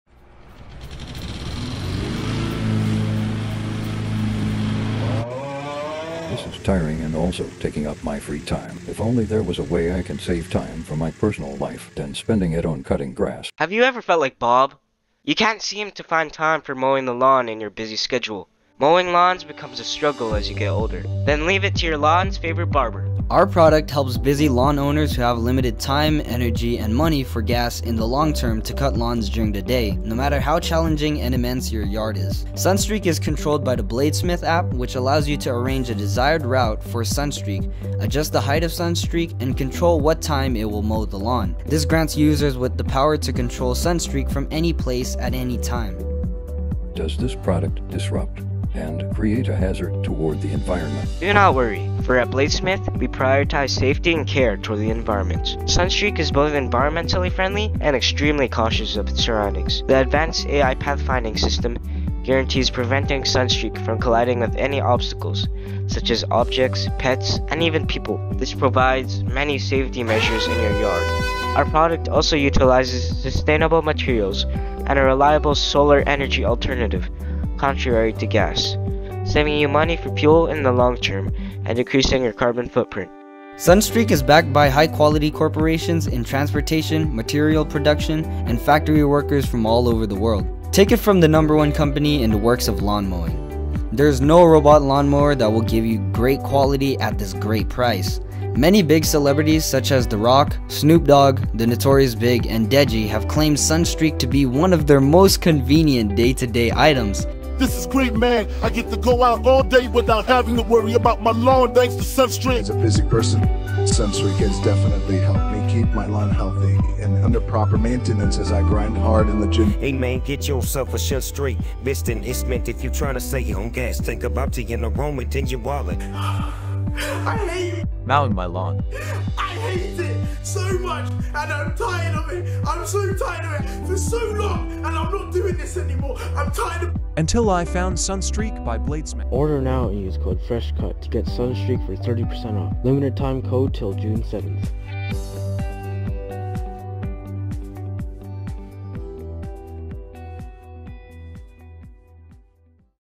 Robot Lawn-Mower Sunstreak Advertisement! (SCHOOL PROJECT: June 1, 2023)
Hello guys it's NjDoger and back in June, we had a social invention convention! Yes not science, social, that's kinda weird but anyways, me and my babft bro Brody decided to make a robot lawn-mower in build a boat. Pretty cool right?